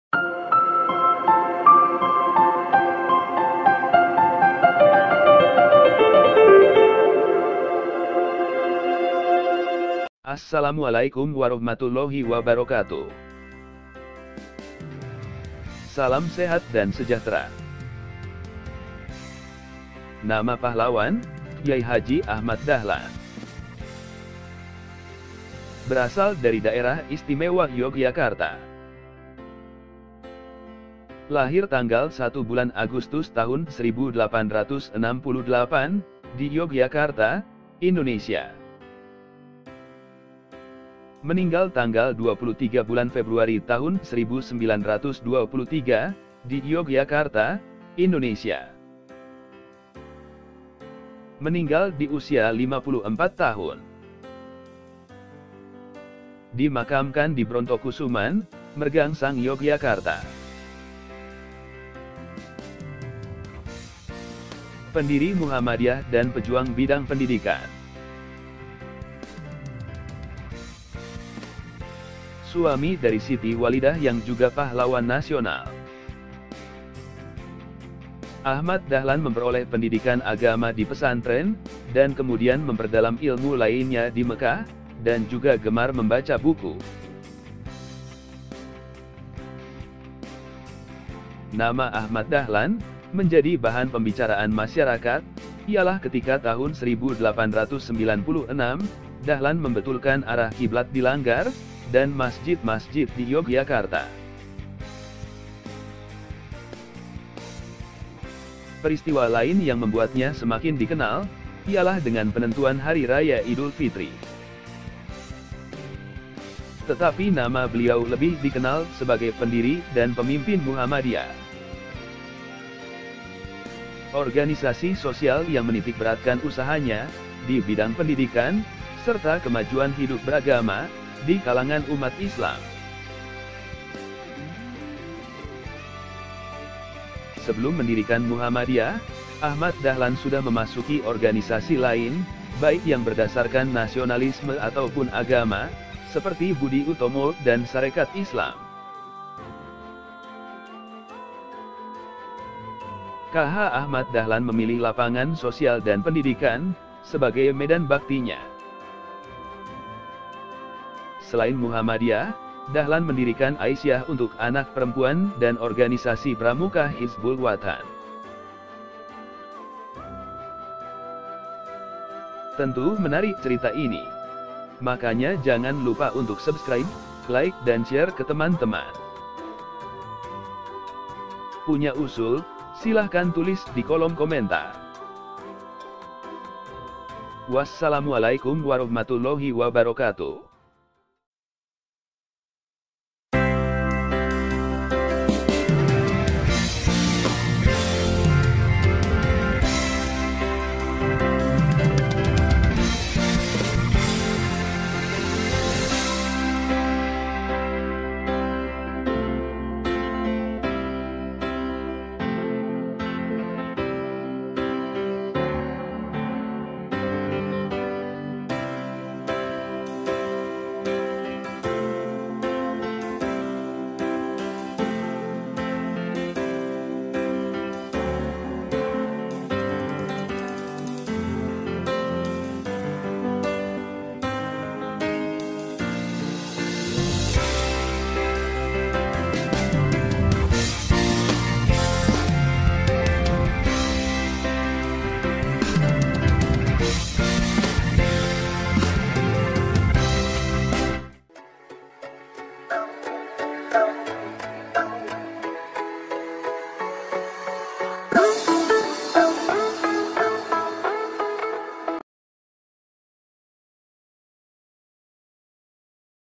Seri Pahlawan-Ku: KH Ahmad Dahlan | (With English Sub-Title) | My Hero: KH Ahmad Dahlan
---KH Ahmad Dahlan---

Berasal dari DIY (Yogyakarta)
 Lahir 1 Agustus 1868 di Yogyakarta, Indonesia
 Meninggal 23 Februari 1923di Yogyakarta, Indonesia
 Meninggal di usia 54 tahun
 Di makamkan Brontokusuman,, Mergangsang Yogyakarta
Pendiri Muhammadiyah dan Pejuang bidang Pendidikan.

Suami dari Siti Walidah yang juga Pahlawan Nasional. Ia memperoleh pendidikan agama di pesantren dan kemudian memperdalam ilmu lainnya di Mekah dan juga gemar membaca buku.

Nama Ahmad Dahlan menjadi bahan pembicaraan masyarakat ialah ketika tahun 1896 Dahlan membetulkan arah kiblat di langgar dan masjid-mesjid di Yogyakarta. Peristiwa lain yang membuatnya semakin dikenal ialah dengan penentuan Hari Raya Id.  Tetapi nama beliau lebih dikenal sebagai pendiri dan pemimpin Muhammadiyah, organisasi sosial yang menitikberatkan usahanya di bidang pendidikan serta kemajuan hidup beragama di kalangan umat Islam.

Sebelum mendirikan Muhammadiyah, Ahmad Dahlan sudah memasuki organisasi lain, baik yang berdasarkan nasionalisme ataupun agama, seperti Budi Utomo dan Sarekat Islam.

K.H. Ahmad Dahlan memilih lapangan sosial dan pendidikan sebagai medan baktinya. Selain Muhammadiyah, Dahlan mendirikan Aisiyah untuk anak perempuan dan organisasi pramuka Hizbul Wathan.